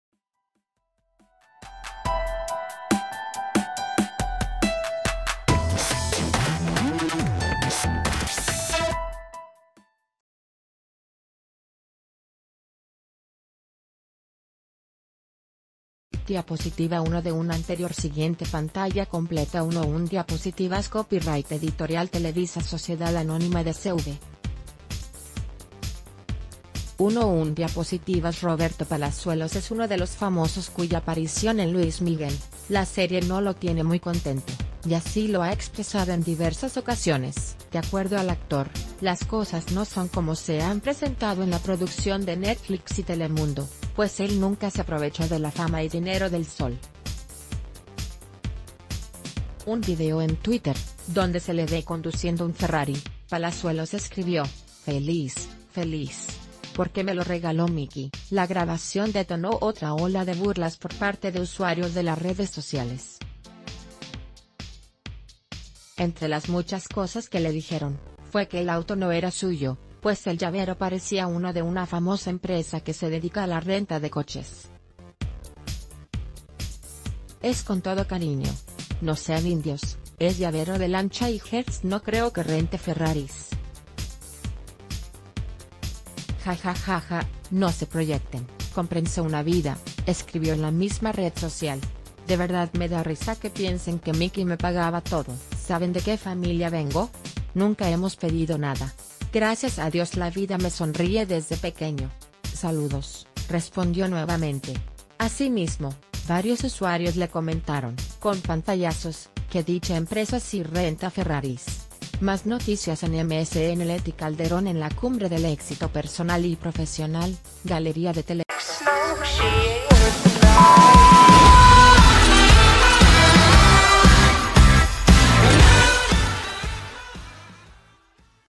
Roberto Palazuelos llama 'indios' a quienes lo critican
Diapositiva 1 de 1 AnteriorSiguiente Pantalla completa 1/1 DIAPOSITIVAS © Editorial Televisa S.A. de C.V. 1/1 DIAPOSITIVAS Roberto Palazuelos es uno de los famosos cuya aparición en Luis Miguel, la serie no lo tiene muy contento, y así lo ha expresado en diversas ocasiones. De acuerdo al actor, las ...